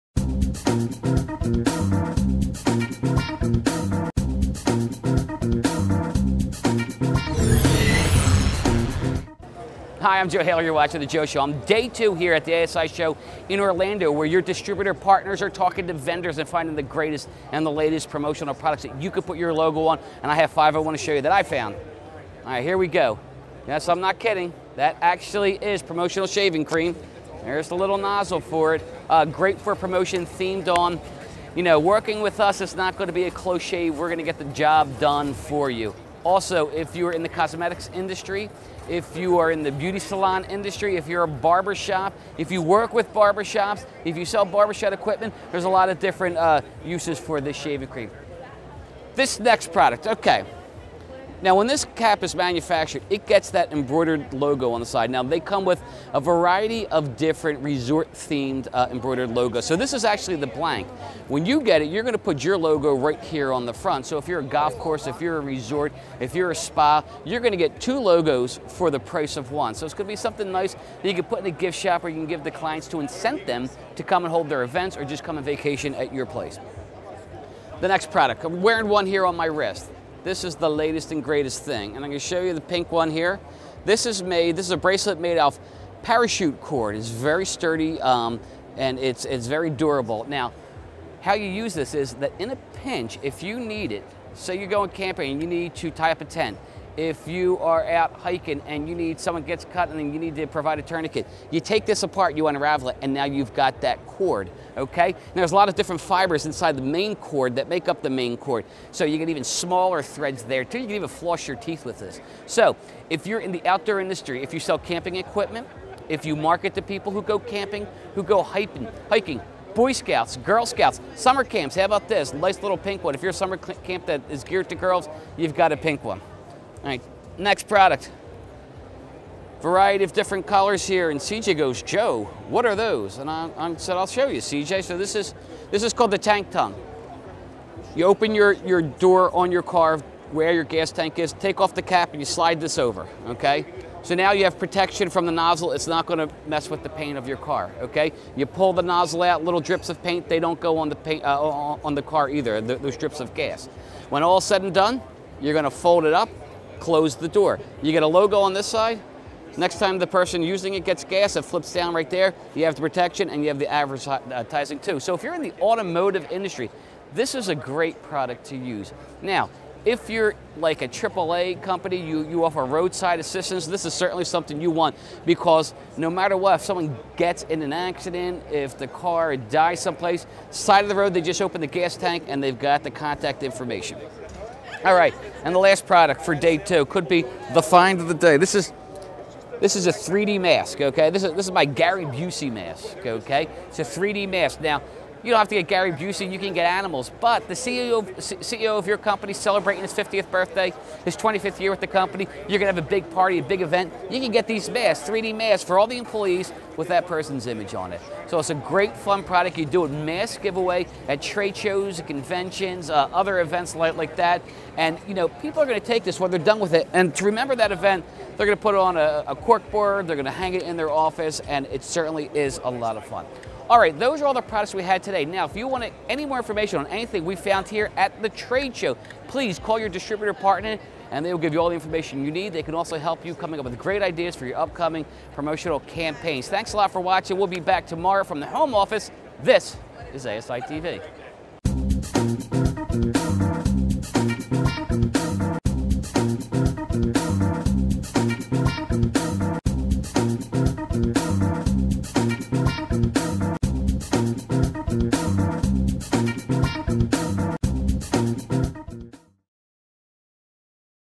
Joe Show Day 2
On this episode of The Joe Show, Joe Haley showcases five unique products he found on the floor of a recent trade show.